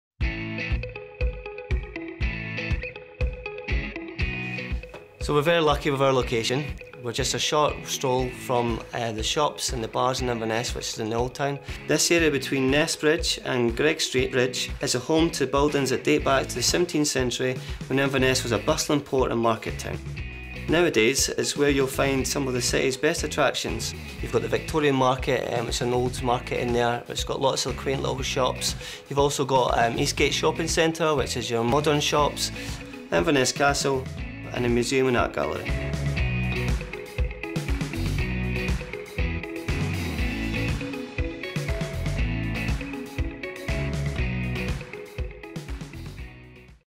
Inverness Shopping – A Local Guide by Premier Inn
Come on a shopping spree with Martin from our Inverness River Ness Premier Inn. Your local guide takes you from Old Town to the Victorian Market and Eastgate Shopping Centre.